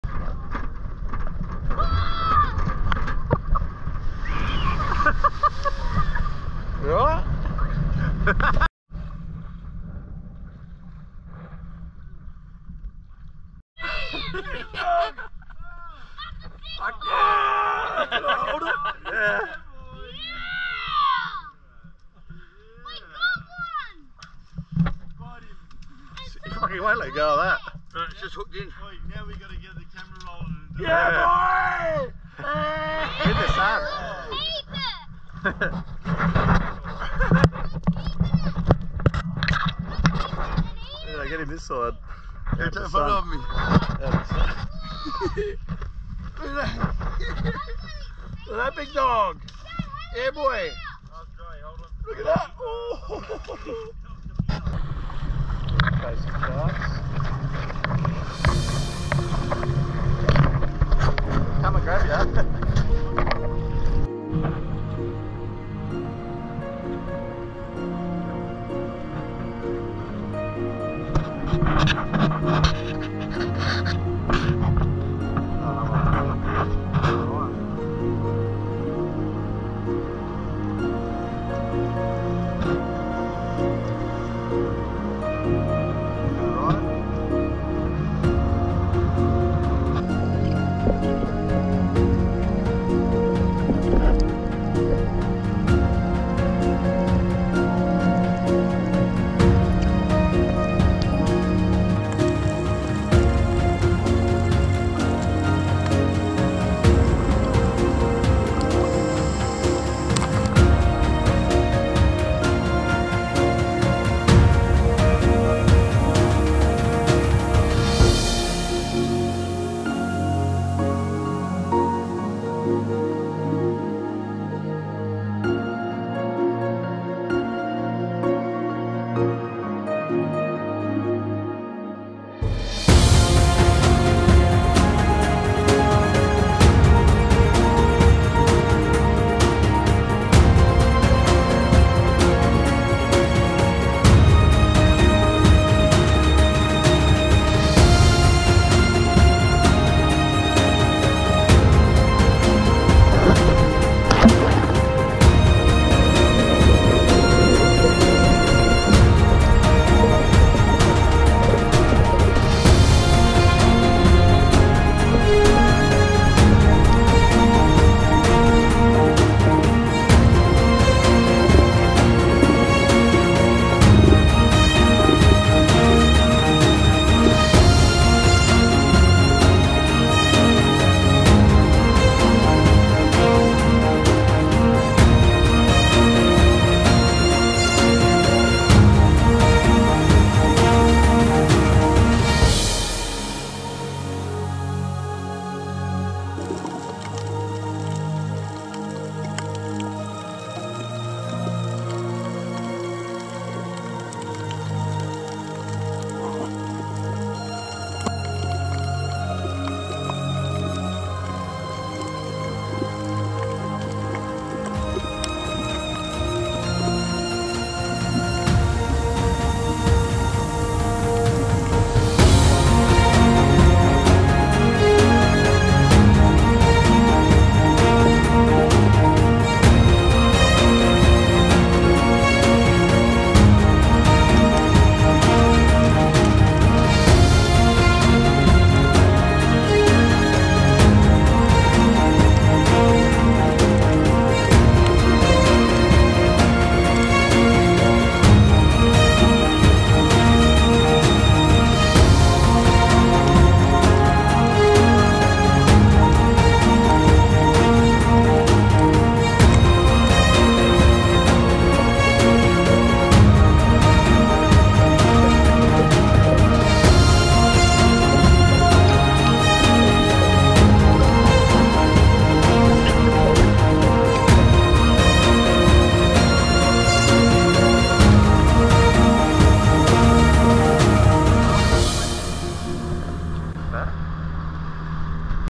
Catching My First Mud Crab
Weather wasn’t the best so decided to go to the mangroves to try for mud crabs, managed to get my first muddy,